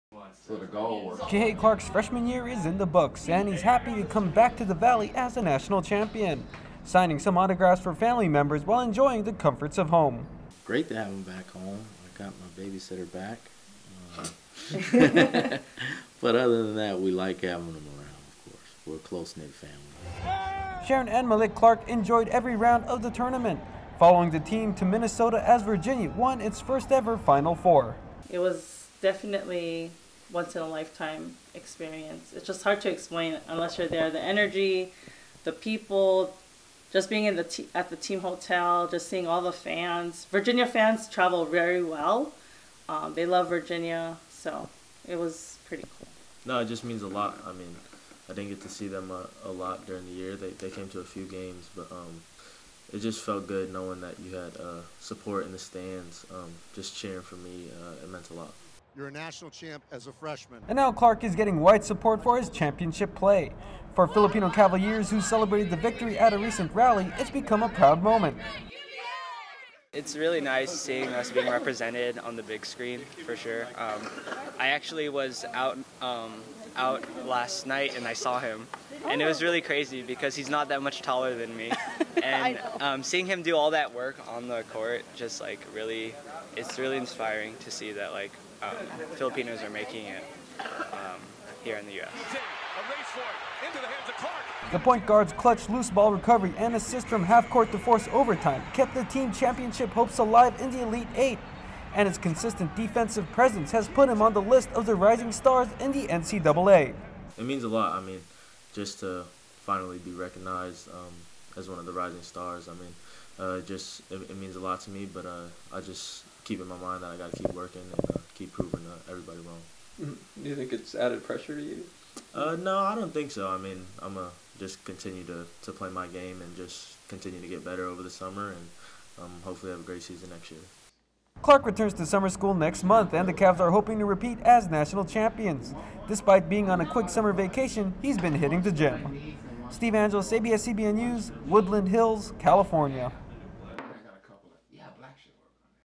NCAA champion Kihei Clark catches up on family time
College freshman Kihei Clark is getting wide support for his recent NCAA championship play. The Filipino in the Virginia Cavaliers recently celebrated his team's victory at a rally.